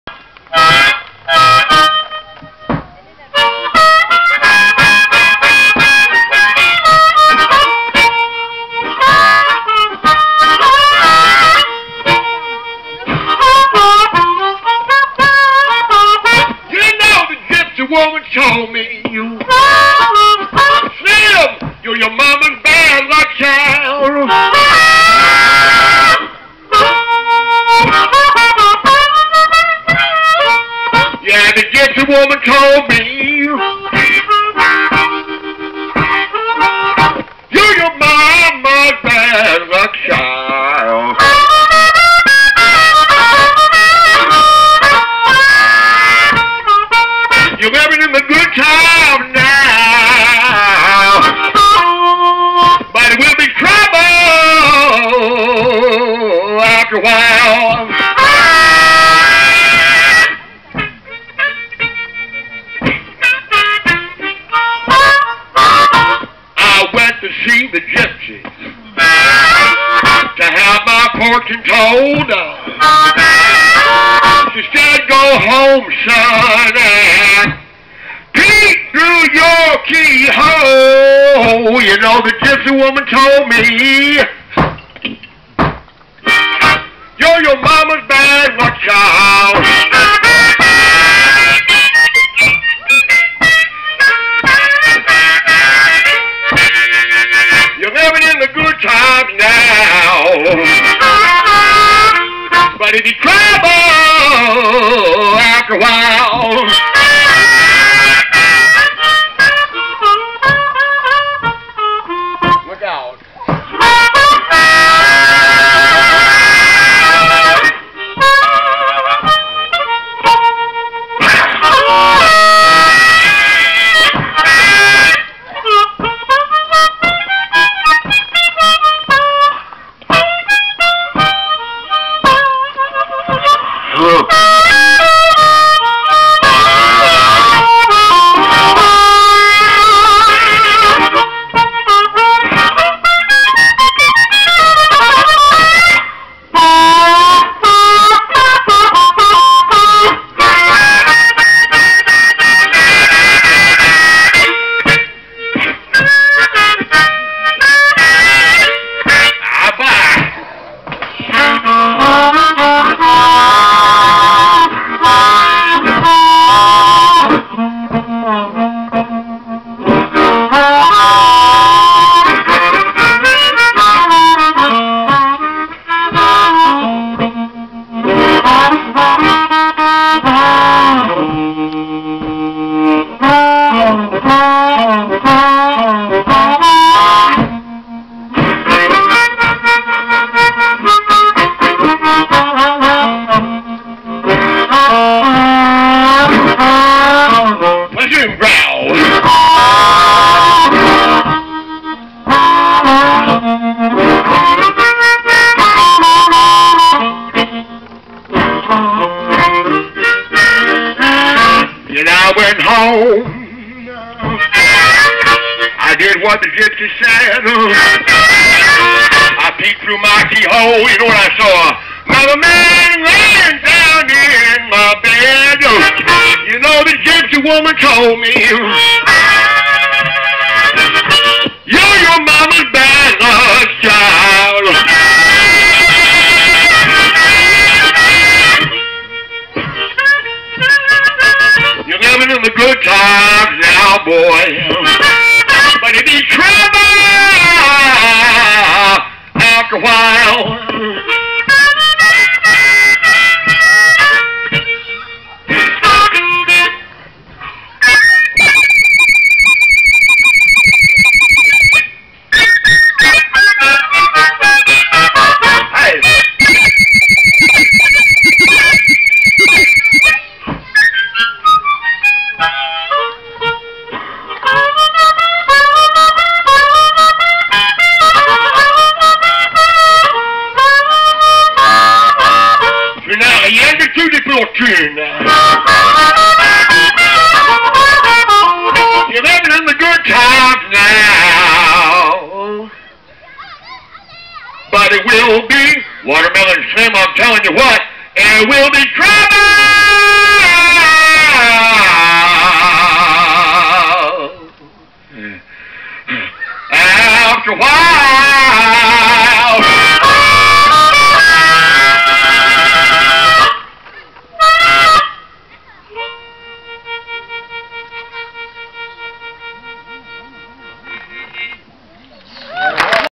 WALTERMELON SLIM / FESTIVAL BLUES PASSIONS 2010
WALTERMELON SLIM / Harmonica Blues - Sur la scène du Festival - Tonic Day le Vendredi 30 Juillet 2010 - Cognac Blues Passions